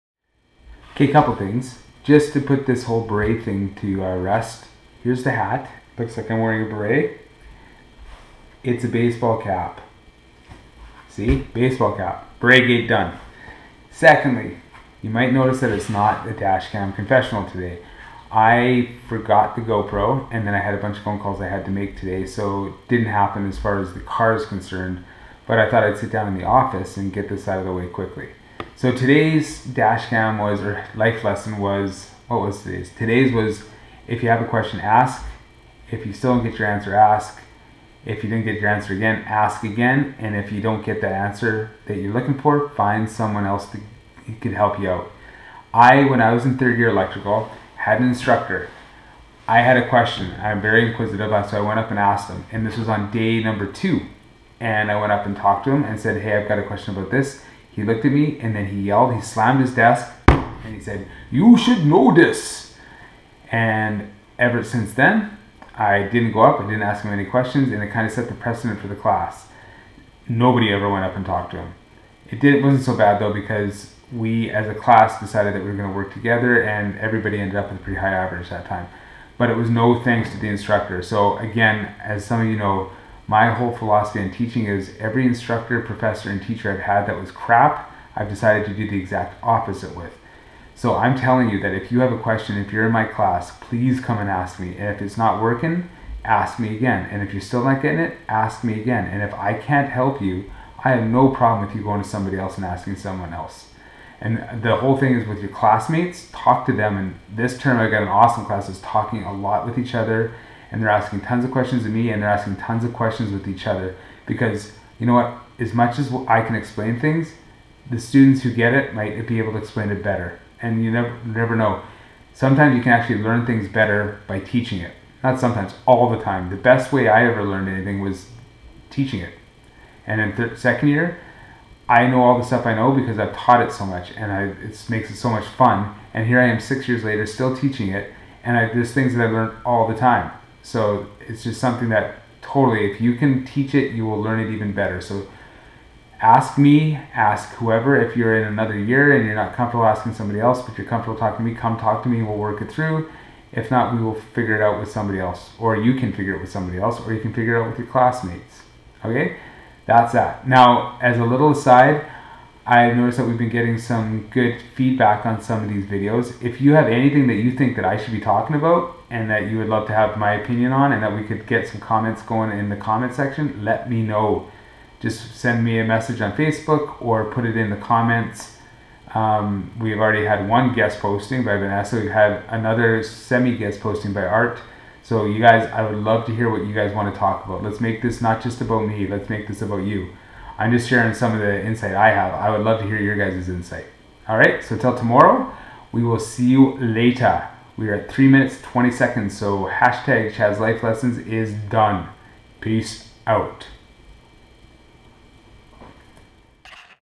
Office cam
Today I forgot my go pro at home so I had to do an office cam. Todays topic was, if you have questions, ask!